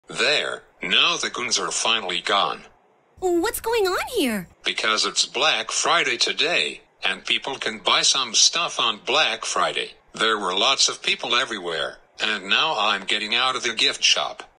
Junior tells Shelly because it’s Black Friday today
Junior Gets Grounded on Black Friday